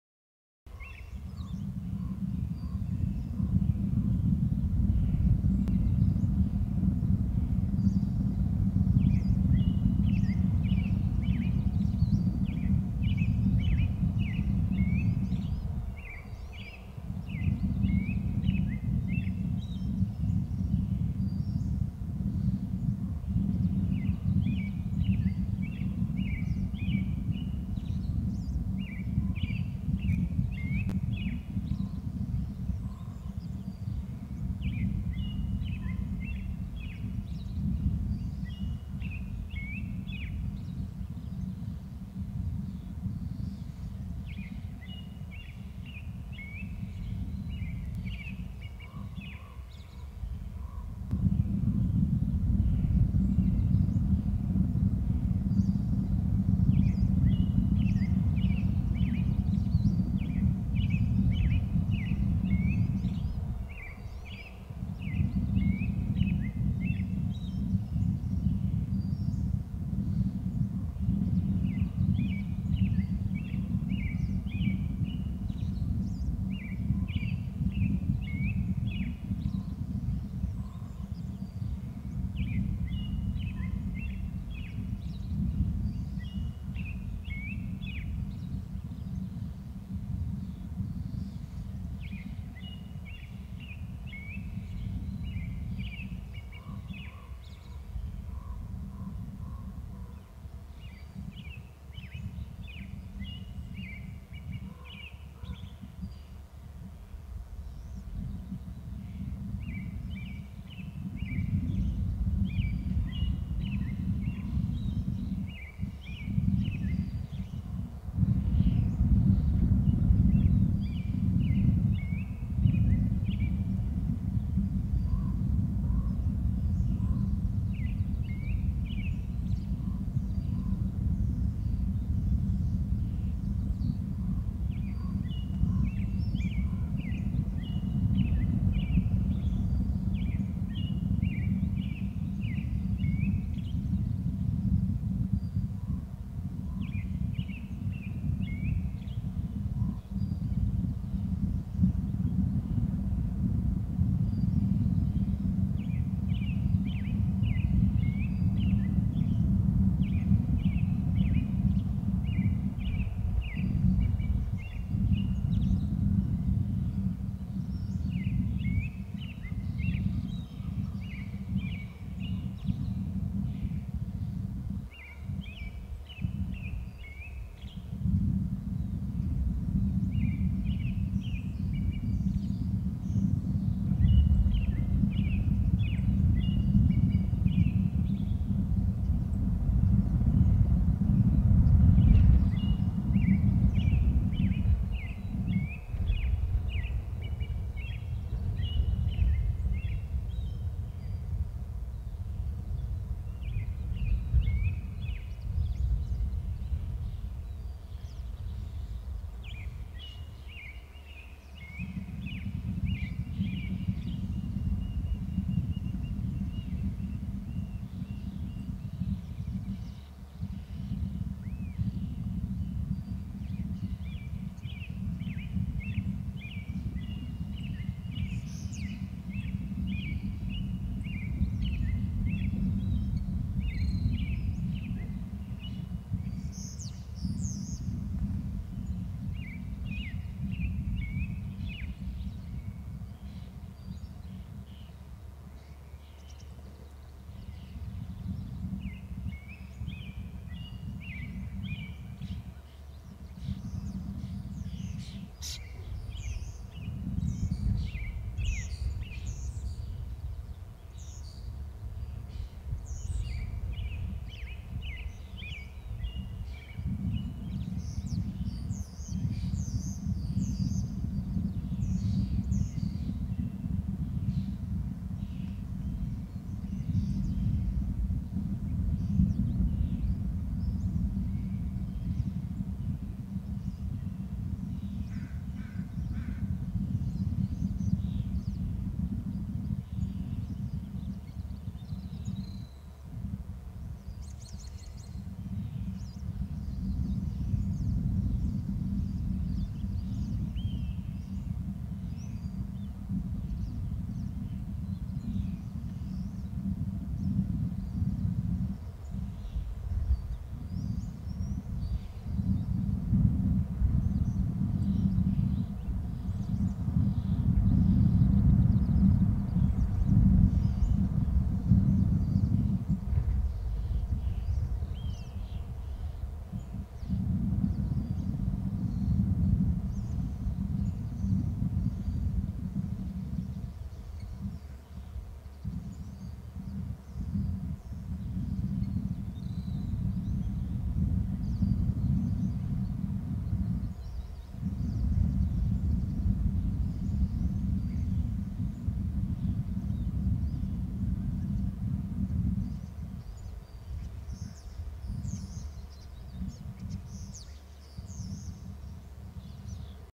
7:02 em in from the right with something 7:09 one to cu 7:10 off
7:02  em in from the right with something,  one up to the cu at 7:09,   7:10 eagle off the cu to the left and the nest eagle is by the stump